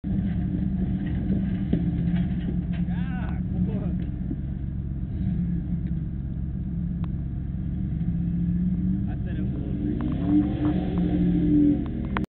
shawn and his bronco at uwharrie
shawn showing a little muscle on some rocks leading up to kodak rock